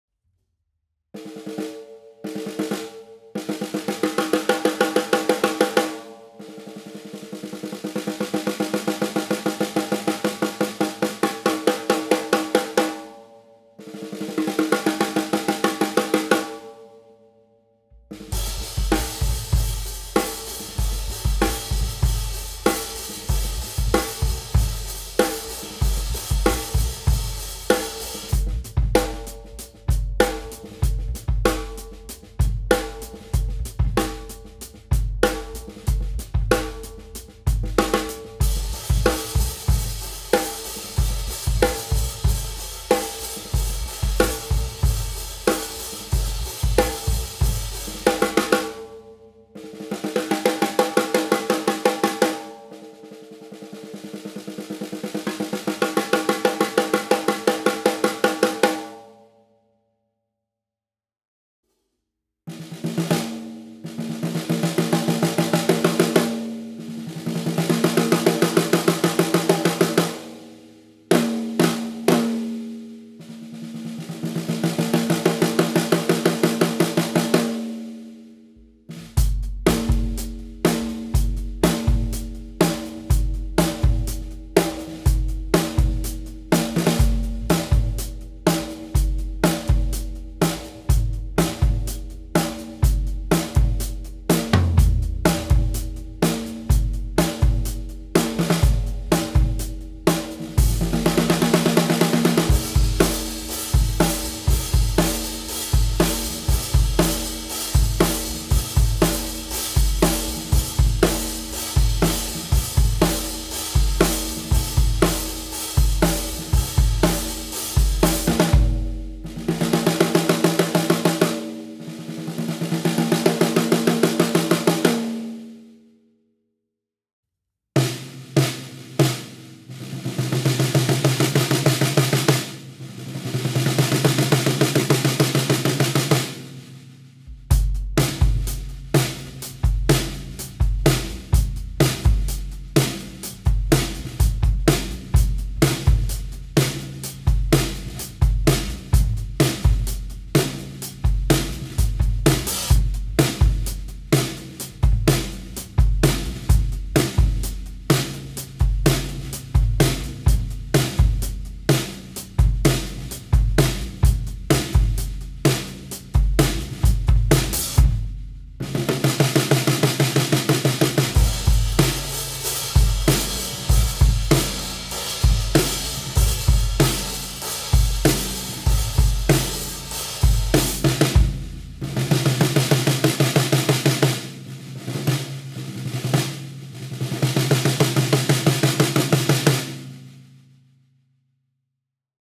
(SOLD) Snares - Blackwood Drum Co. 7x14 Seamless "Aged" Brass Snare Drum
Blackwood Drum Co.

Drum: Seamless Aged Brass

Size: 7x14

Features:

Beaded Seamless Brass Shell
Tube Lugs
Trick Throw
Triple Flanged Hoops